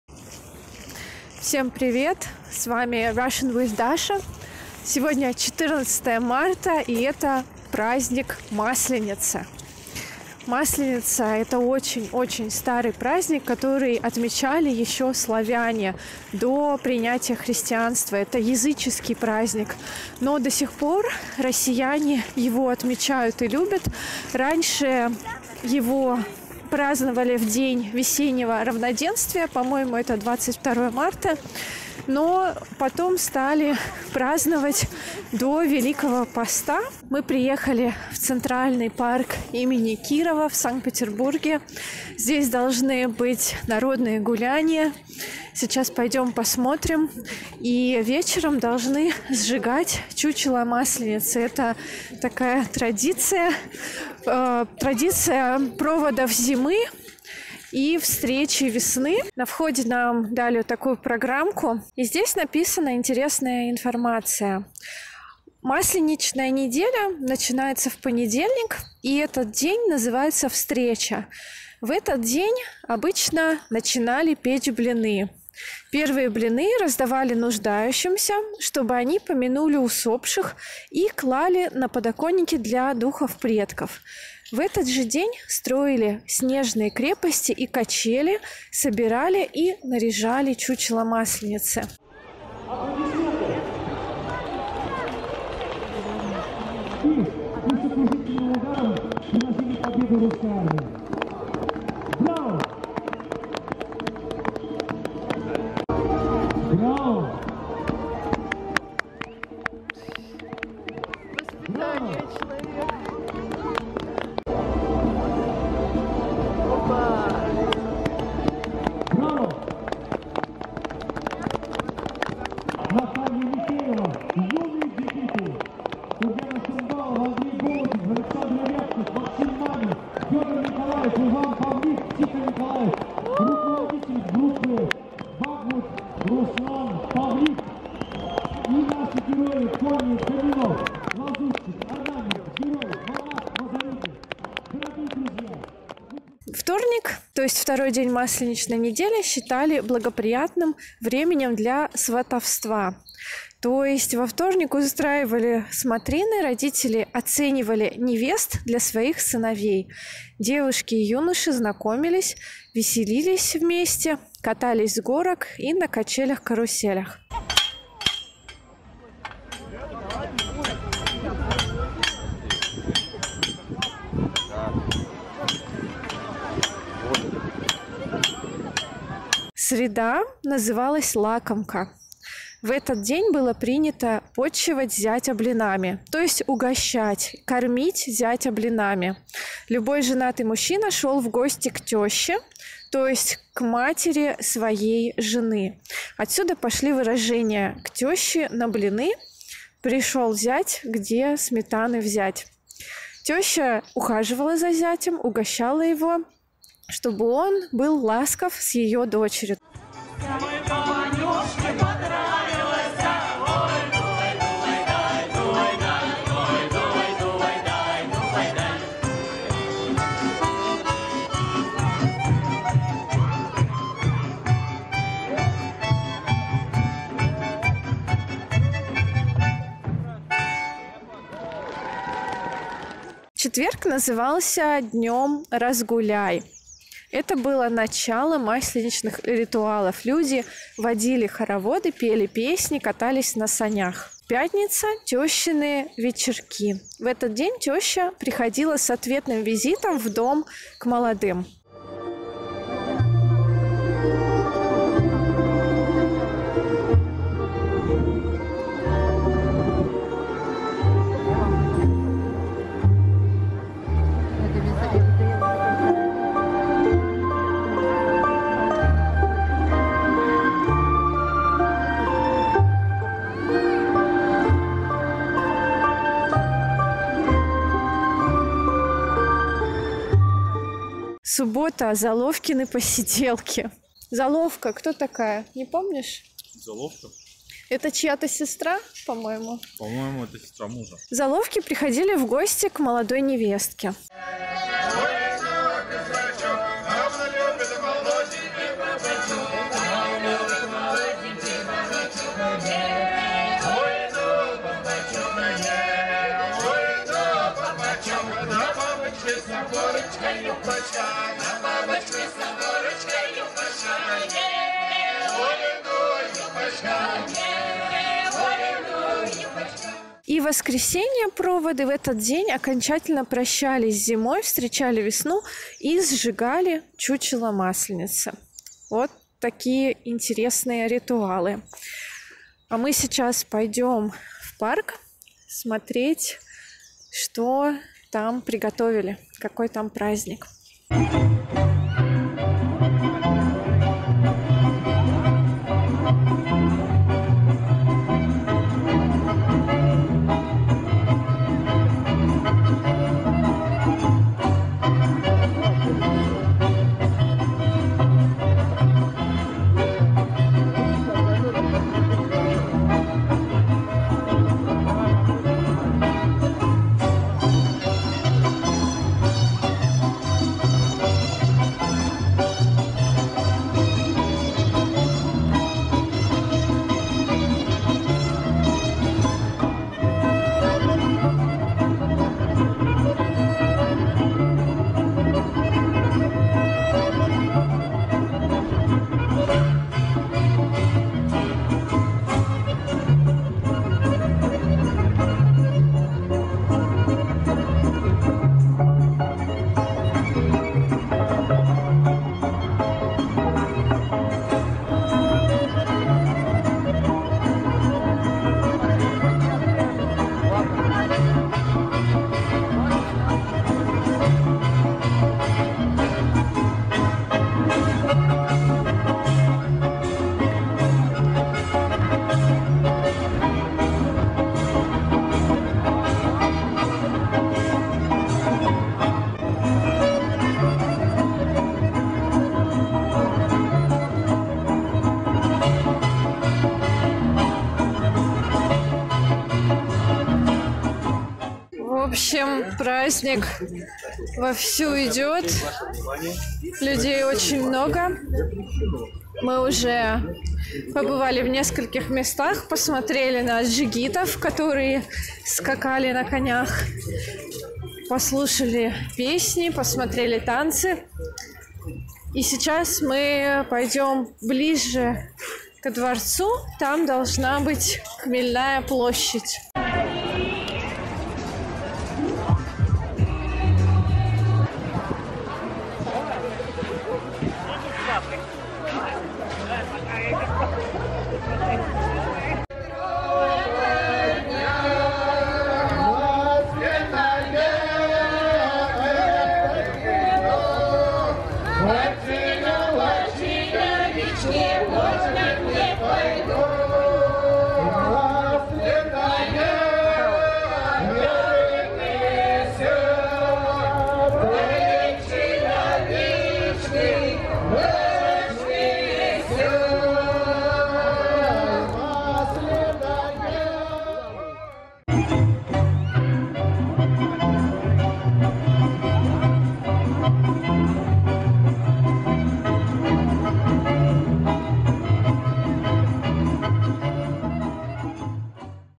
Maslenitsa in Russia. Pancake day with folk dances and songs. Learn Russian with subtitles B1-B2
Russian Maslenitsa in Saint Petersburg. A fun holiday for the whole family. Let's see how it's celebrated. Learn Russian with subtitles. PDF transcriptions of my videos and podcasts with accented text and English translations